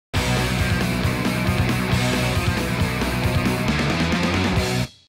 Cool Cleveland Motion Graphics Intro (5 Seconds)
Tim and Eric-inspired motion bump for CoolCleveland.com. Made by David Wester. The man in the intro is Thomas Mulready of CoolCleveland.com.